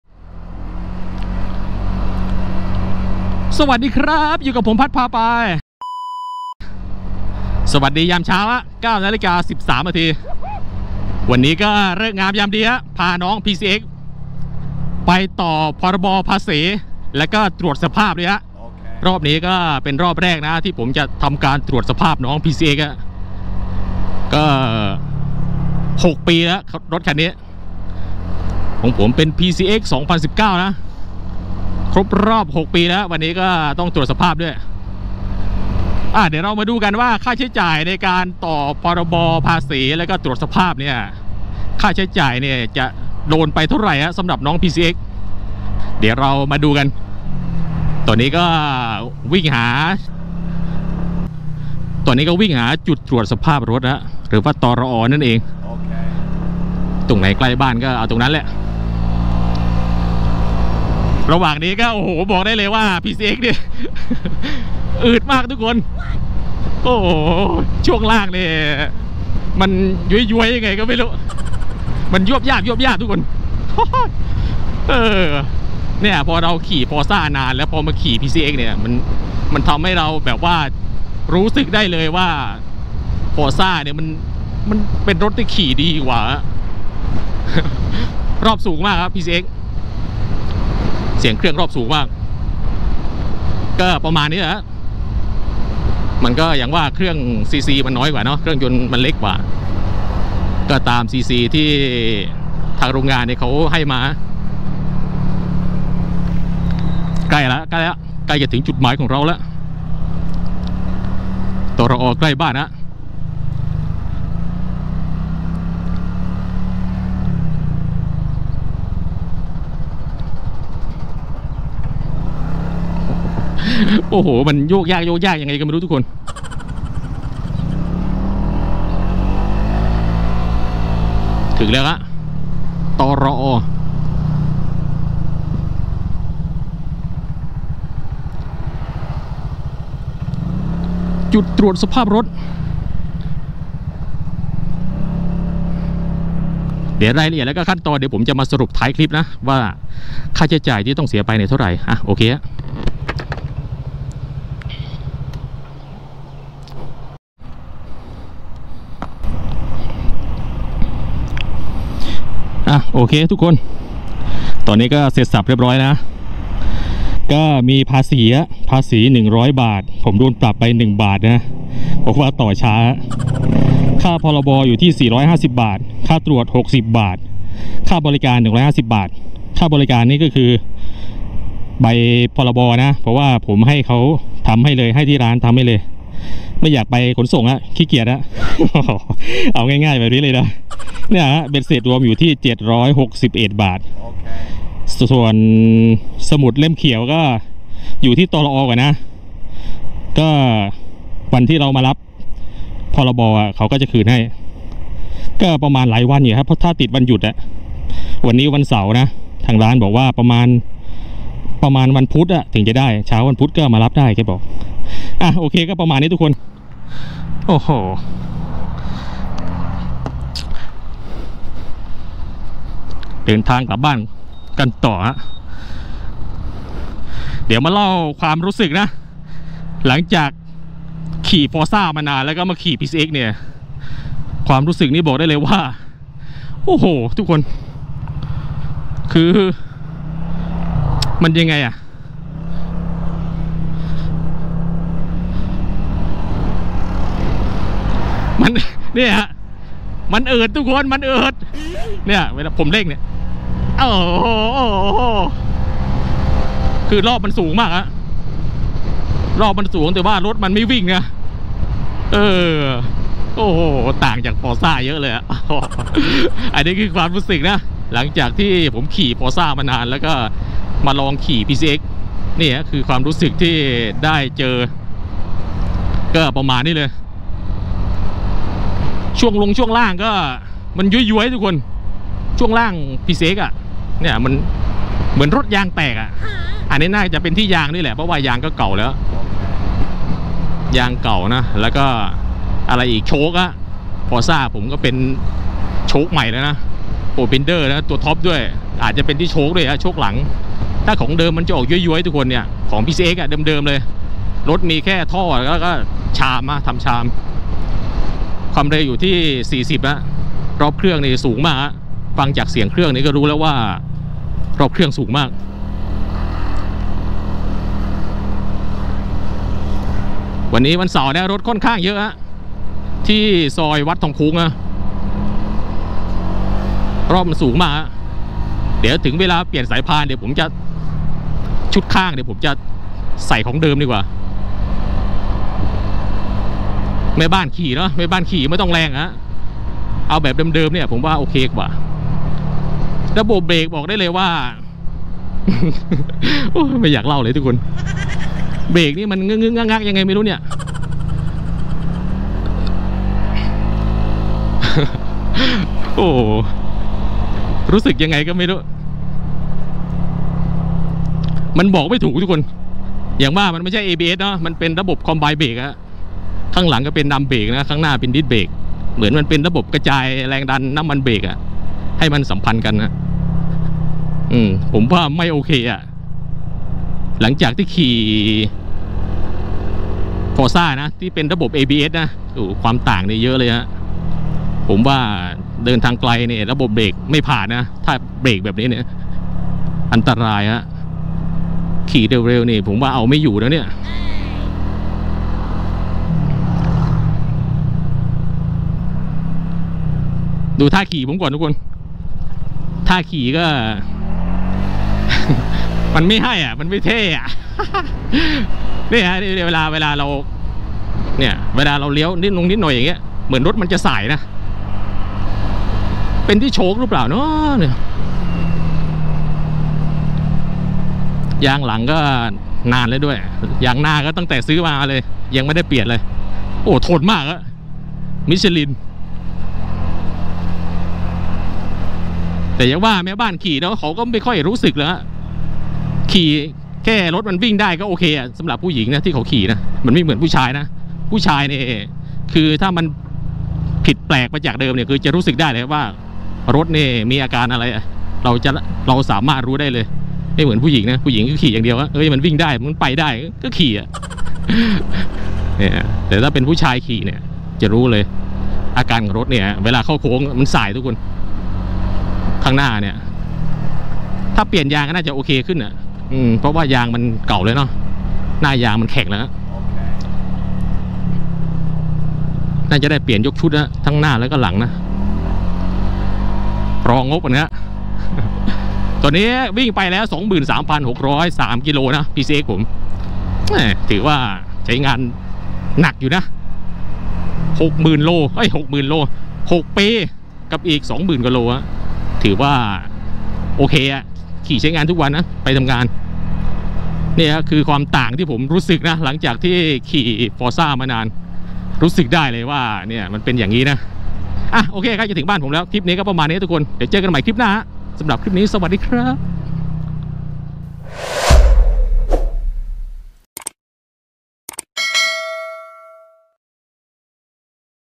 เมื่อผมขี่ Forza 350 มานาน แล้ววันนี้ต้องมาขี่ Pcx 150 แหละนี้คือความรู้สึก ขี่ Pcx ไปต่อ พรบ.
🛵 ขี่ไปเล่าไป กับบรรยากาศระหว่างทาง Forza 350💨

❤️🙏ขอบคุณที่เข้ามารับชม🙏❤️